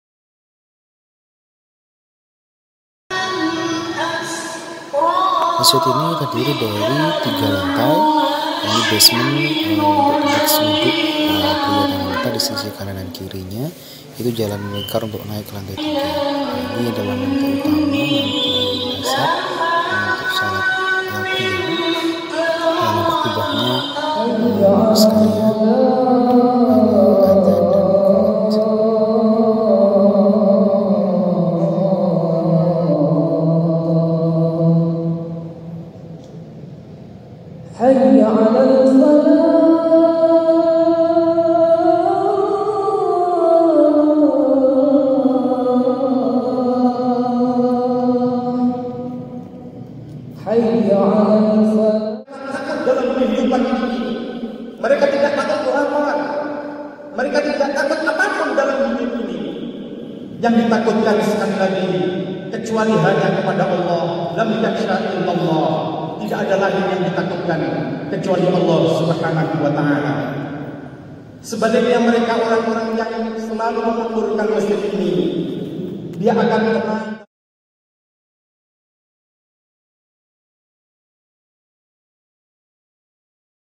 Sholat Jum'at Perdana Masjid Al Hikmah UNEJ
menjelang datangnya bulan Ramadhan tahun ini, Masjid Al Hikmah kampus Unej hadir dengan tampilan baru.